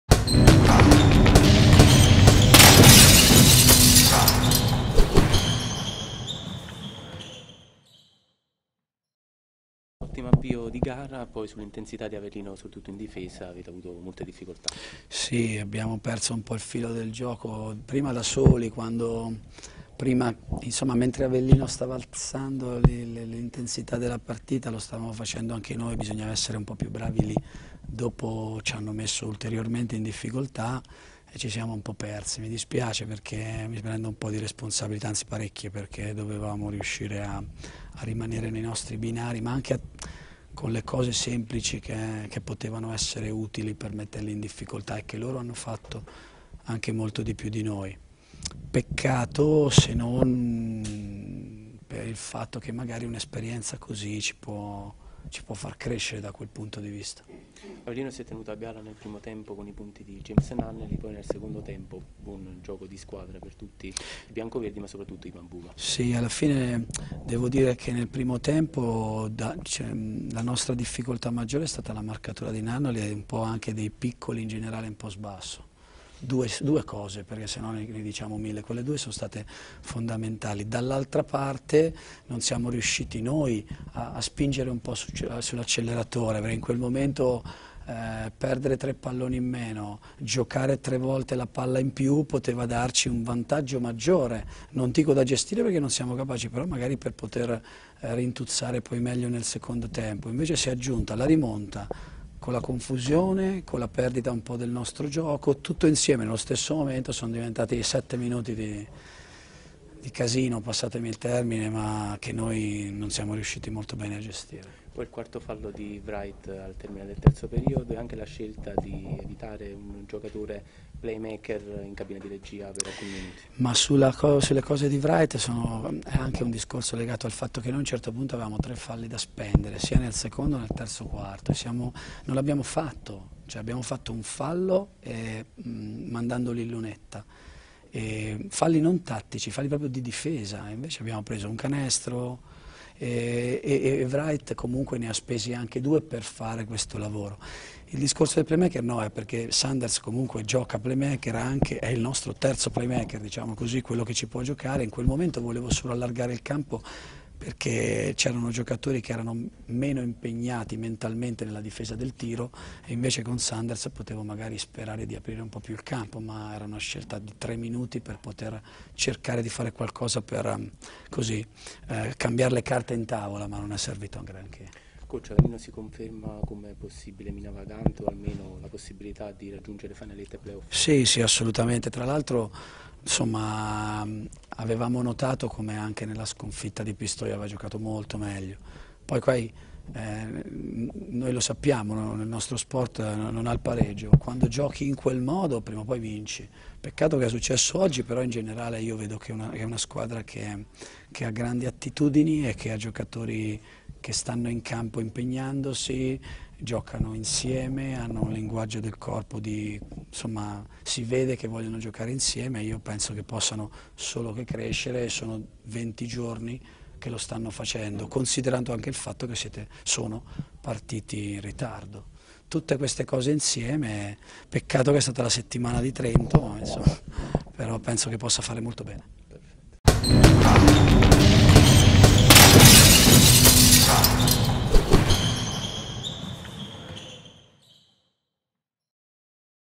Sidigas Avellino - Dolomiti Energia Trento, le parole di coach Buscaglia
Il commento sulla sconfitta al PaladelMauro